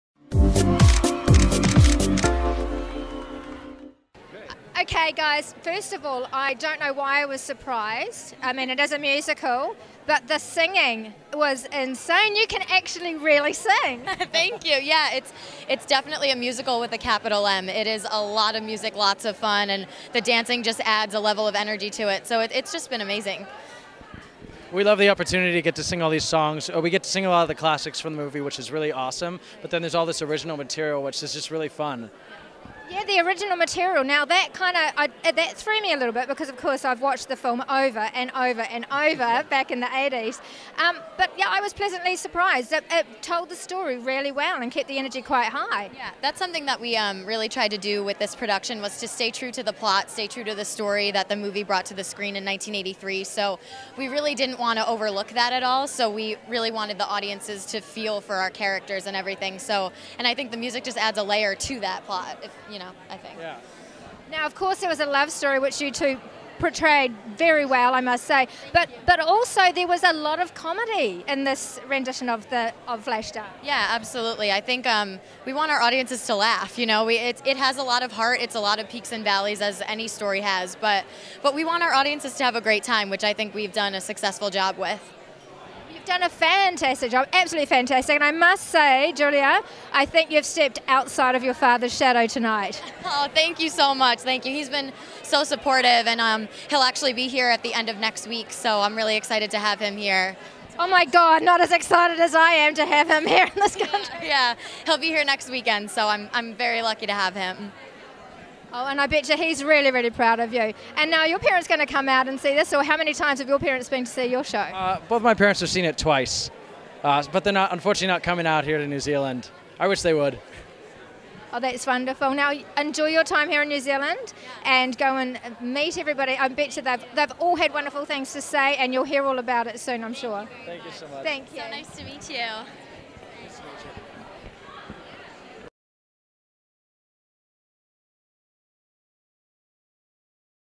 Flashdance - Interview with Julia Macchio and Ryan Neal Green
Redcarpetnz.tv meets the stars of Flashdance: The Musical!

New Zealand's Online Entertainment Channel!
FILM | FASHION | MUSIC | TV
"We roll out the RED Carpet & bring the Party to you!"